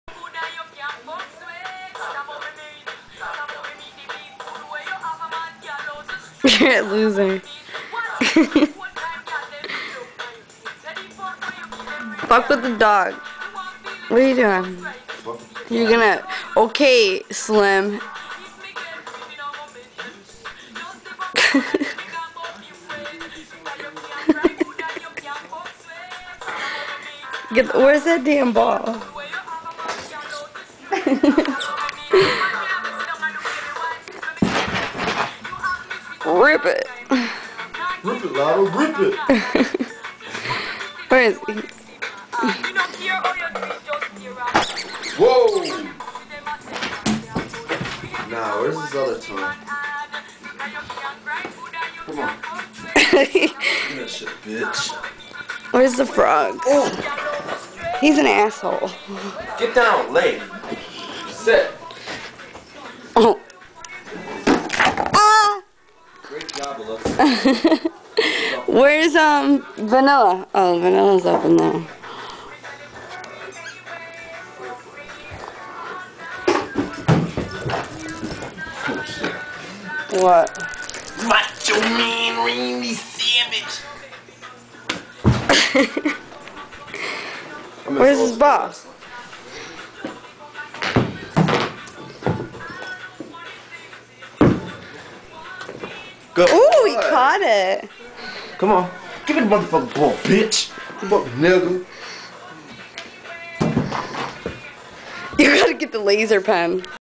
My man playen w/ our animals
Trey n I messen round and him teasen them w/ the lazer pen